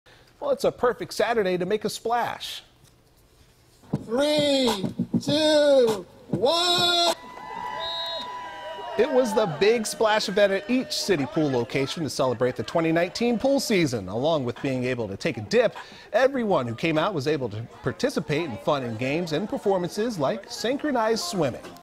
City Hosts Big Splash Event Celebrating 2019 Pool Season
The City of Baltimore hosted its annual Big Splash Event to celebrate the beginning of the 2019 pool season.